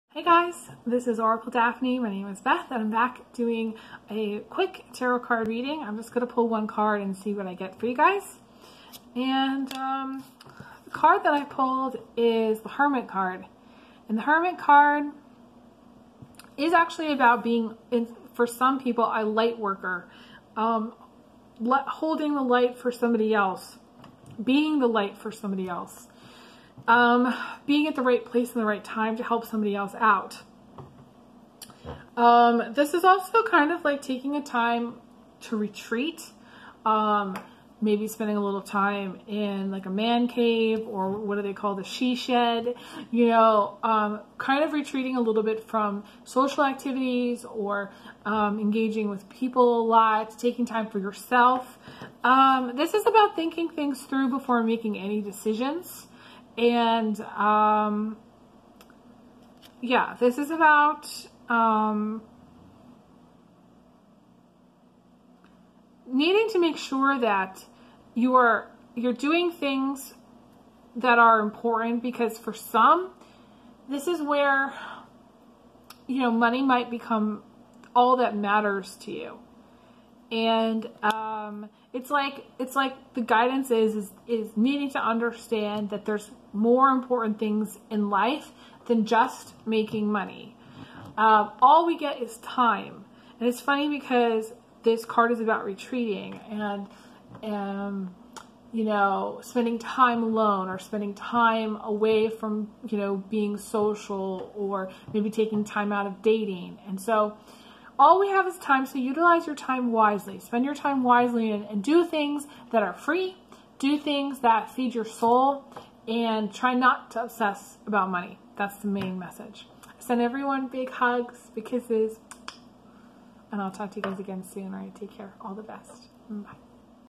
super short (1) tarot reading
This is a super short tarot reading where I pull a card and channel the messages that come through for it.

XOXO
Oracle Daphne 🦋